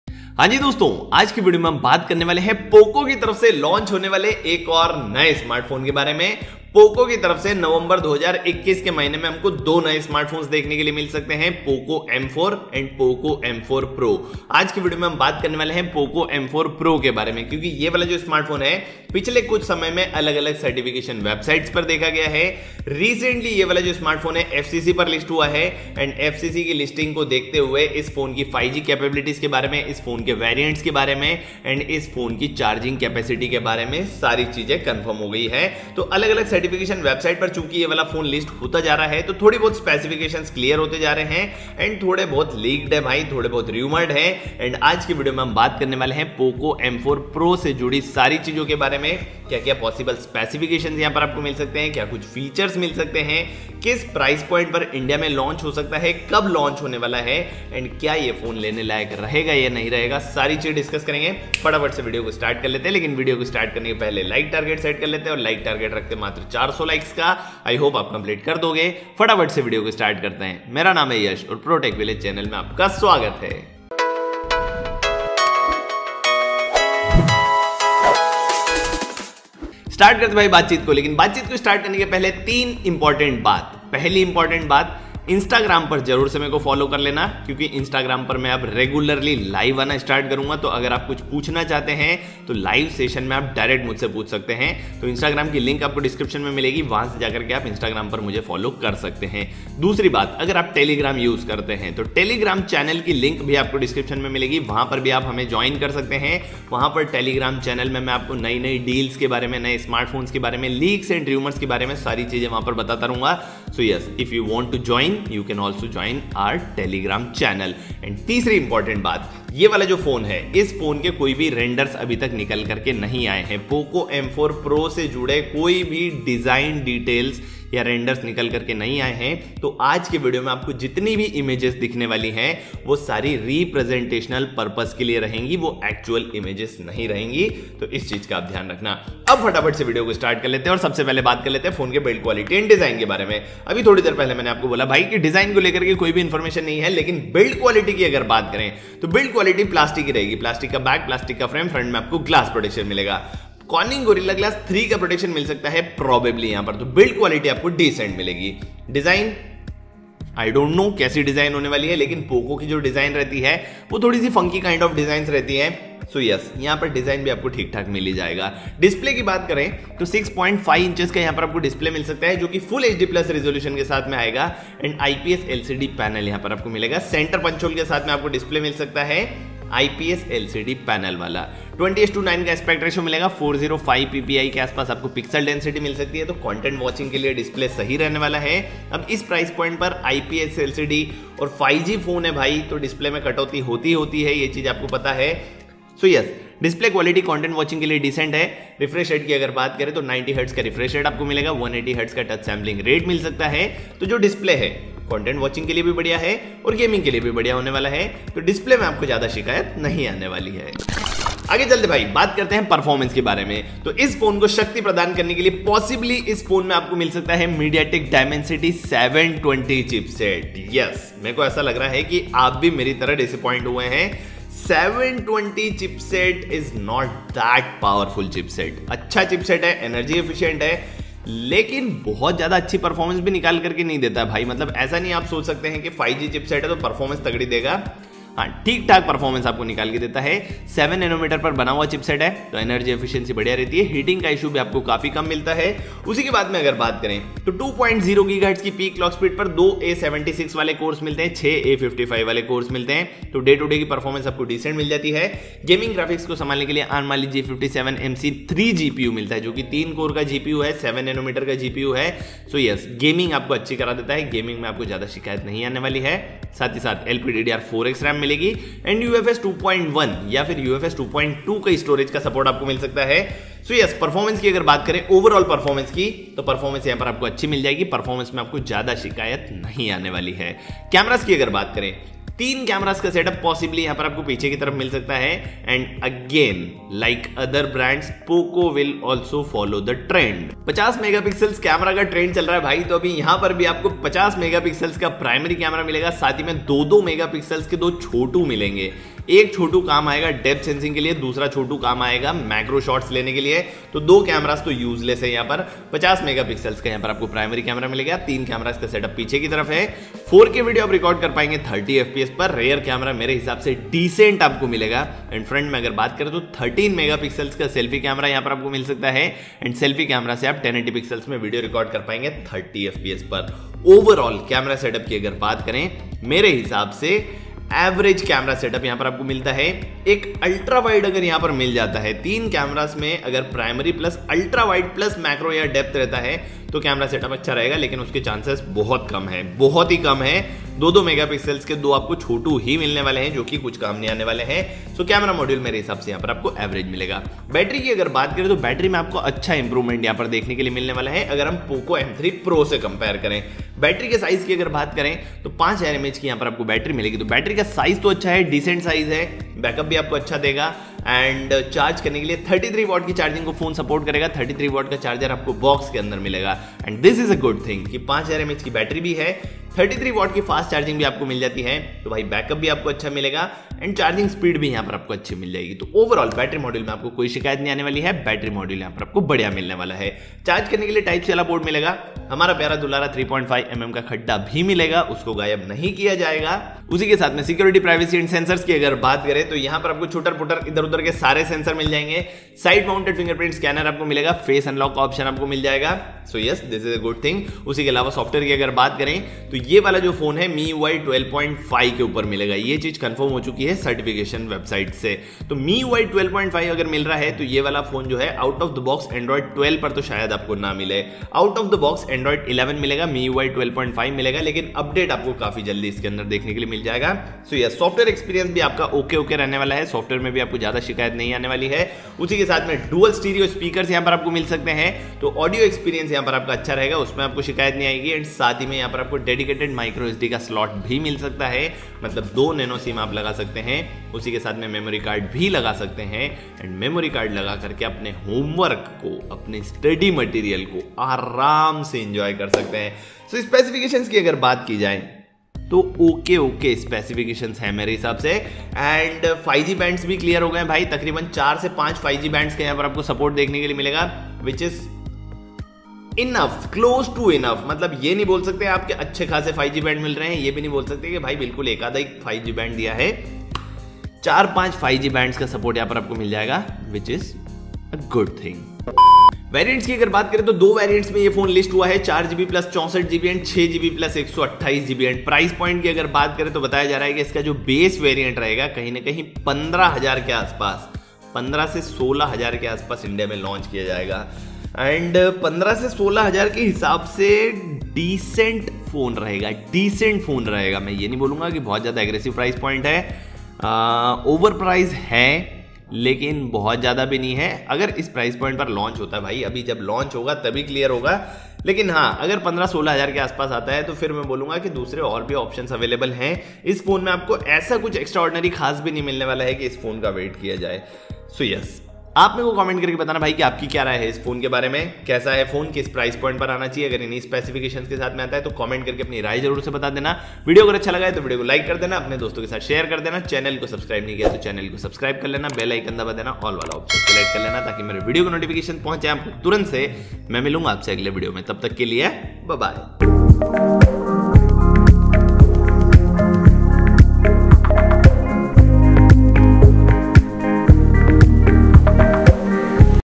POCO M4 PRO 🔥🔥| DIMENSITY 720 at RS.15,999..? [HINDI]
Hello Guys,

About this video:

Hello Guys,

Is video me humne baat ki hai POCO m4 PRO k specifications and features ki. I hope apko ye video pasand aayega.

Thanks